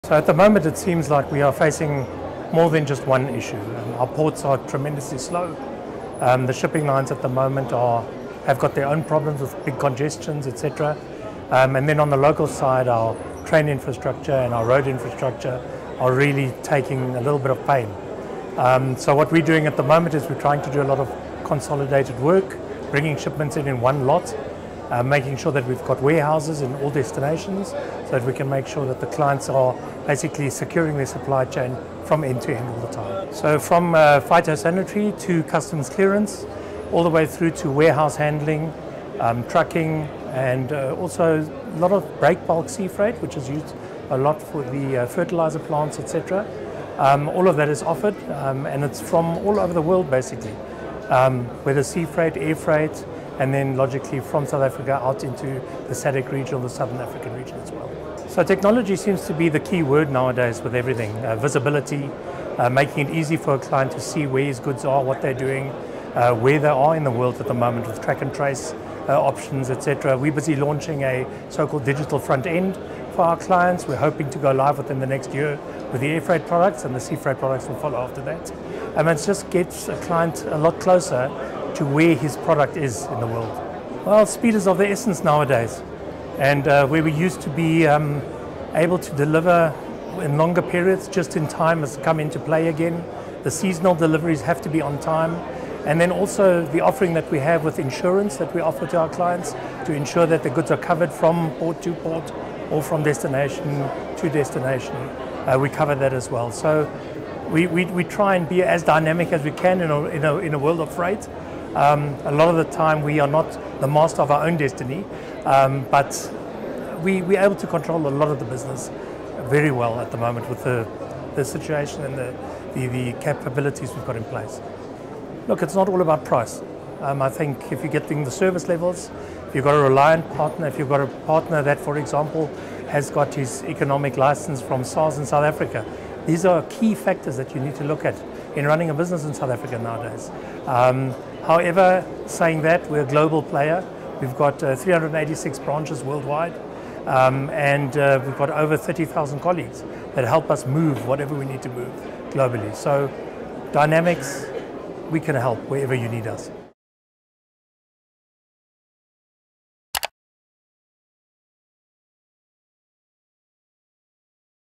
Dachser South Africa at Nampo 2024
Engineering News discusses how family-owned logistics provider Dachser South Africa is innovating to address the challenges that agricultural logistics face in South Africa with MD Detlev Duve at Nampo 2024, which was held from May 14 to 17 near Bothaville, in the Free State.